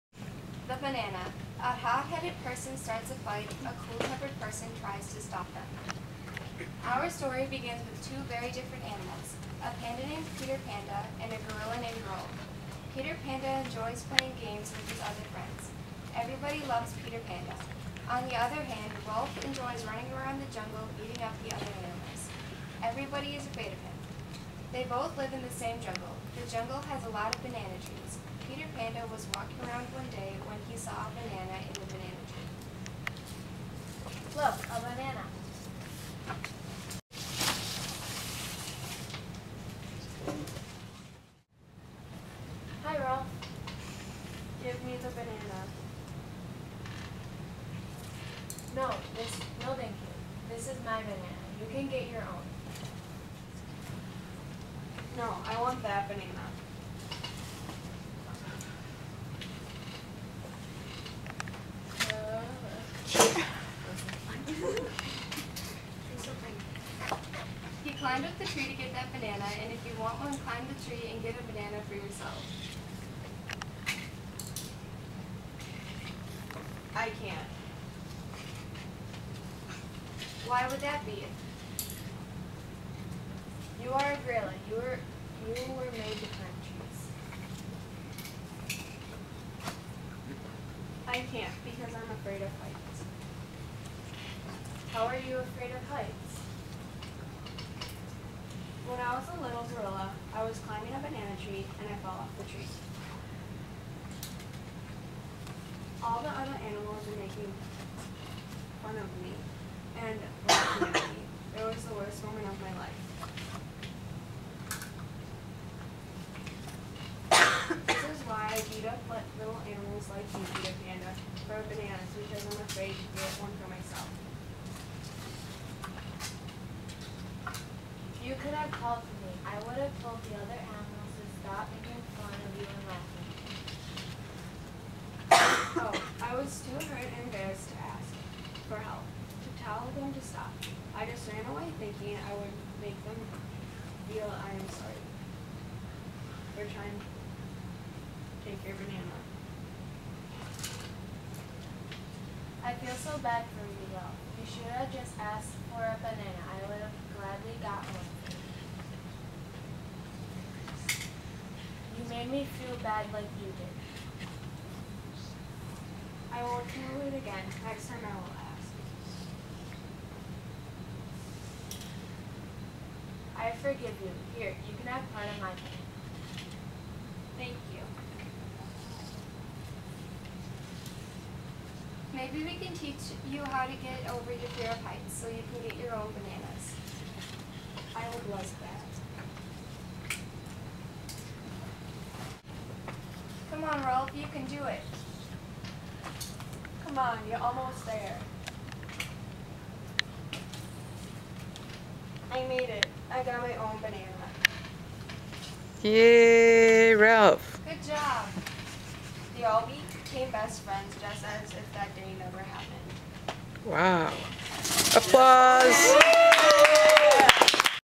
Ralph and the Banana ... !
Eighth grade art elective students combine literacy and visual art to produce puppet shows for children.  Students wrote the scripts, created the stages, sets, props, characters, and then performed them for their class ...

Bravo!